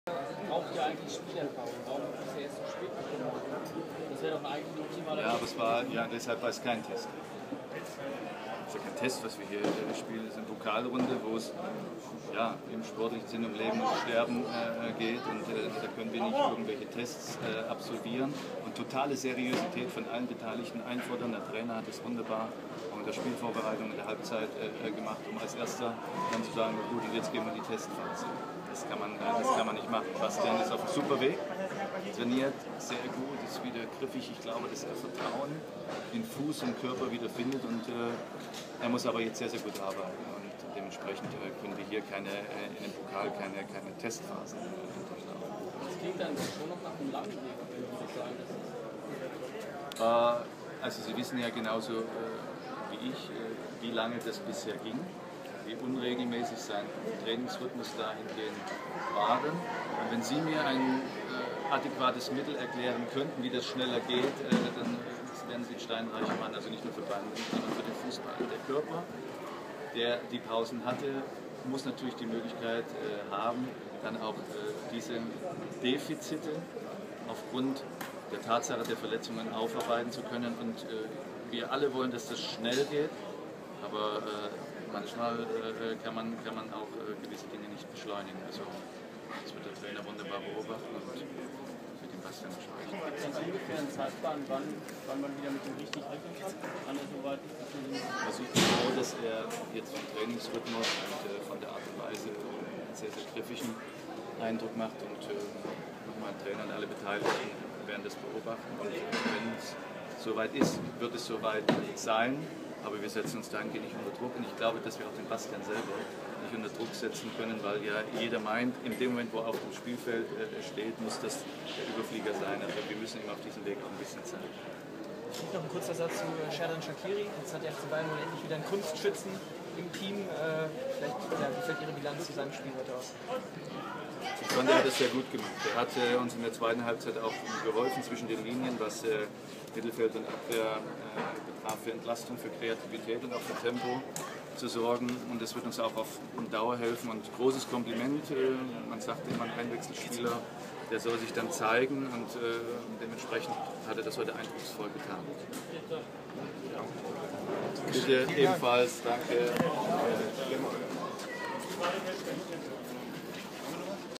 Matthias Sammer über Bastian Schweinsteiger
Nach dem 4:0-Sieg in der ersten Runde des DFB-Pokals spricht Bayerns Sportvorstand Matthias Sammer im Interview über das Comeback von Mittelfeldregisseur Schweinsteiger. PNP-Sportredakteur Andreas Lakota war dabei.